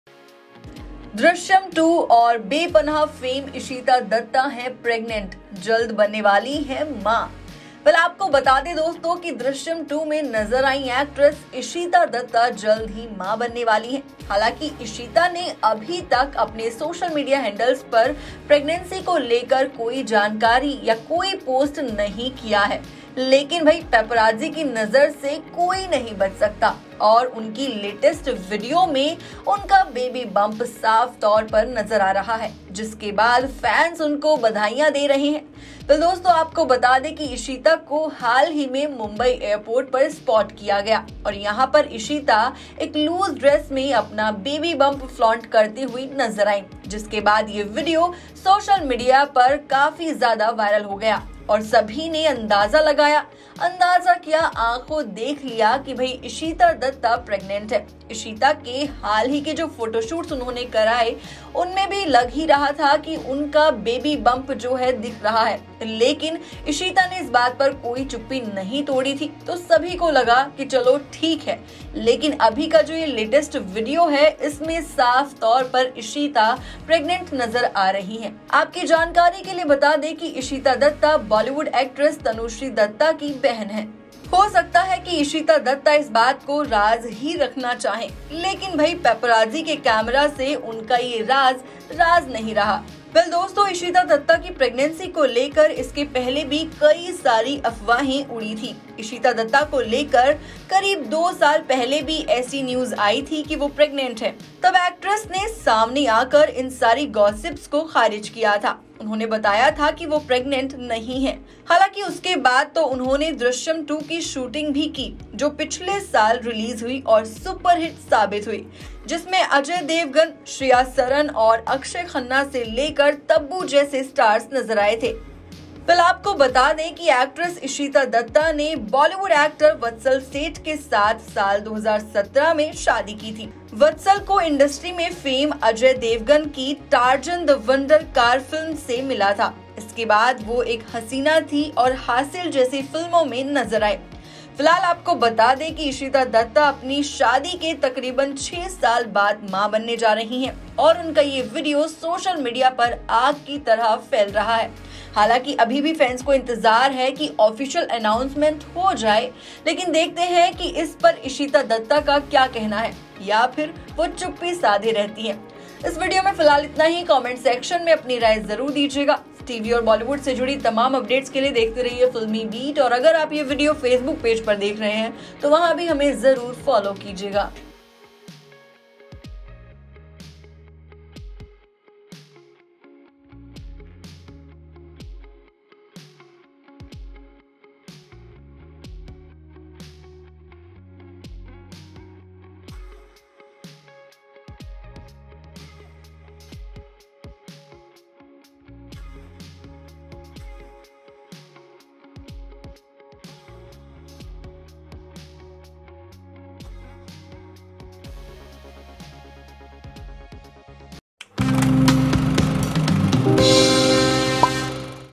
Drishyam 2 Fame Ishita Dutta हैं PREGNANT; Ishita Dutta का दिखा Baby Bump, Pregnancy Video Viral
Ishita Dutta, who was last seen in Drishyam 2, is expecting her first child. The actress, who has been a part of several TV shows and films, was spotted today at the airport and flaunted her baby bump. Ishita Dutta and Vatsal Seth are going to be parents after approx 6 years of their marriage. Watch the video to know more.